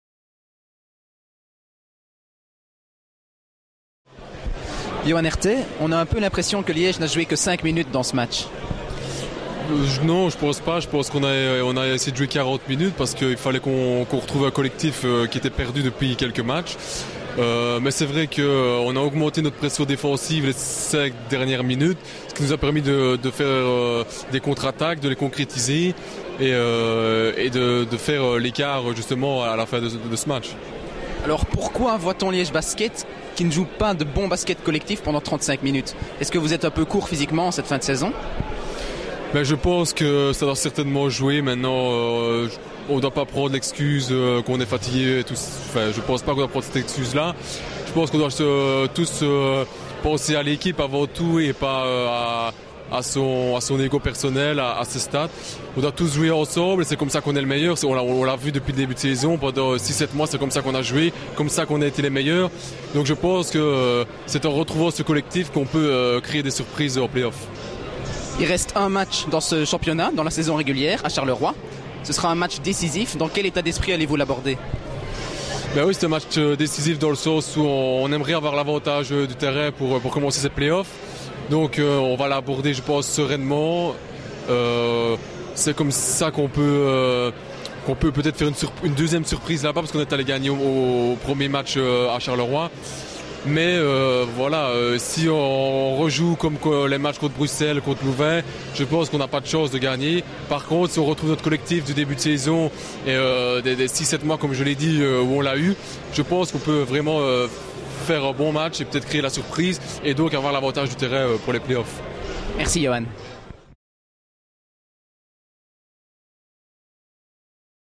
Liège Basket - Verviers Pepinster: 75-65 - Interview de Yoann Hertay - 7/05/14
Interview de Yoann Hertay #10.